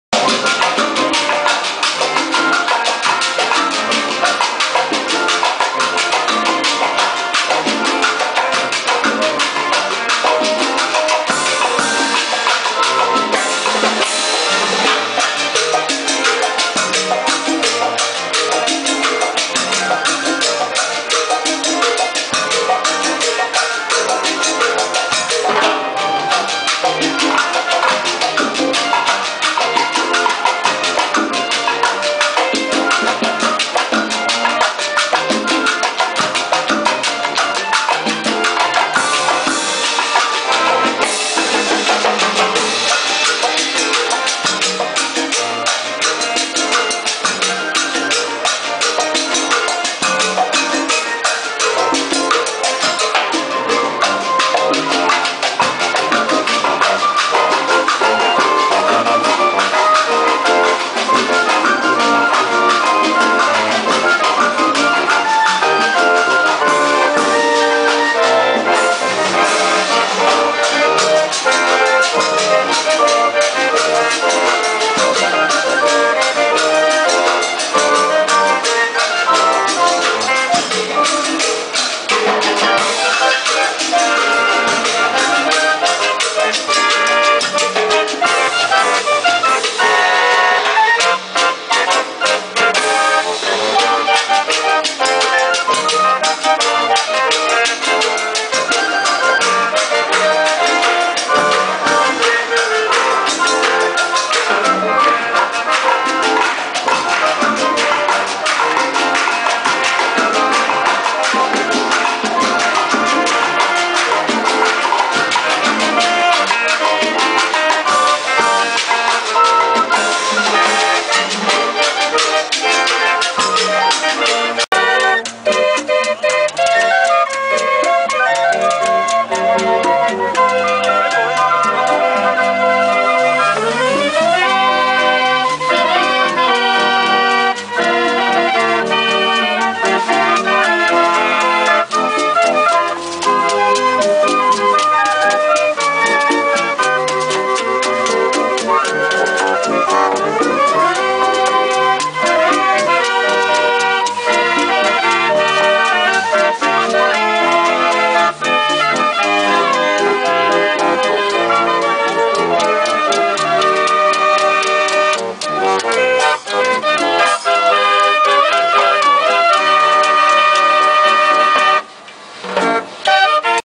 Draaiorgel op Cuba. Cubaans draaiorgel
Het eerste gedeelte van het filmpje is door mijn vader (Joop Walter) op Cuba opgenomen met zijn fotocamera. Dus niet al te beste kwaliteit.
Het tweede gedeelte in Haarlem.
Maar wel heel erg leuk.
Draaiorgelboeken worden daar gemaakt van oude verpakkingsdozen.
Daar heeft hij ook nog foto's van. Zodra ik kans zie, en weet hoe dat moet, zal ik die ook op You tube publiceren.
Hij was op Cuba in een draaiorgelfabriek toen de draaiorgels schoongemaakt werden.
Er kwam een rondborstige Cubaanse schone met een emmer water en een mop.
De mop werd flink ondergedompeld in het water en het draaiorgel kreeg de volle laag water.
Onvoorstelbaar.
Cubaanse draaiorgels kunnen in tegenstelling tot de Nederlandse orgels, wel tegen water.